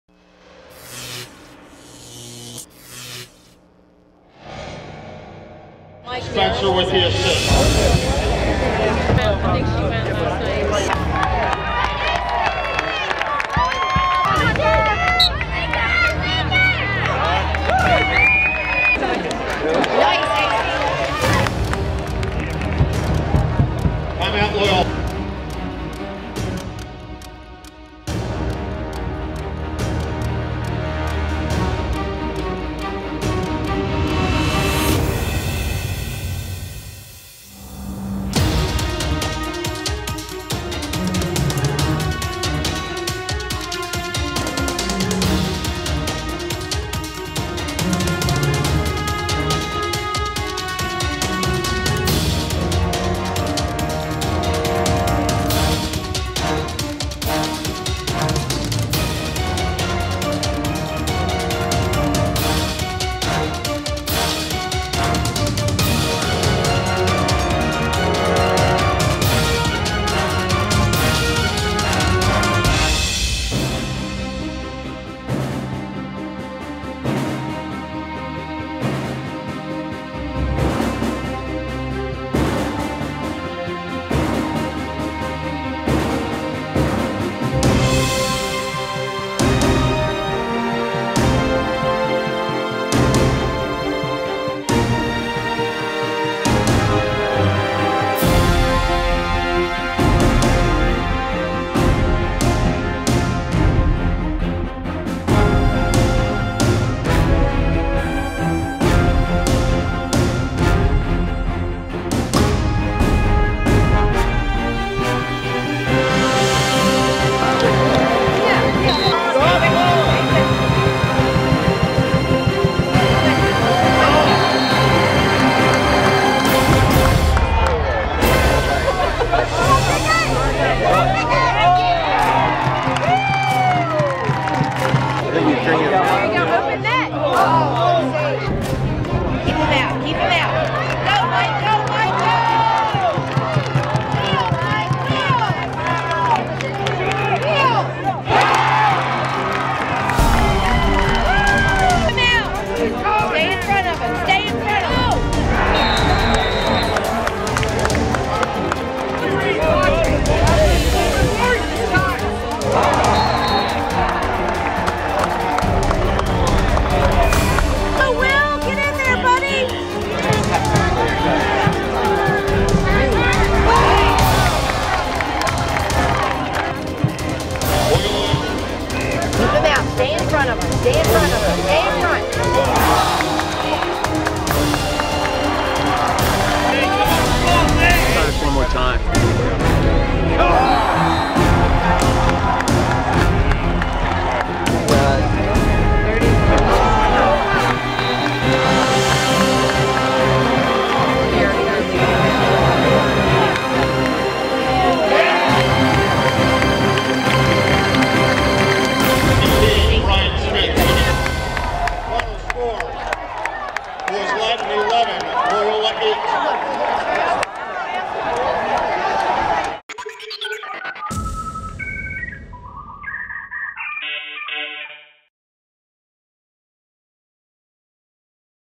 Boy's Latin vs Loyola Blakefield | 2014 Lax.com High School Highlights
- In a rematch of last years MIAA "A" Championship, the countries top ranked boy's lacrosse team, Boy's Latin, traveled to Loyola Blakefield.  Led by a four-point performance from Shack Stanwick, Boy's Latin held onto their undefeated record in 2014, defeating Loyola, 11-8.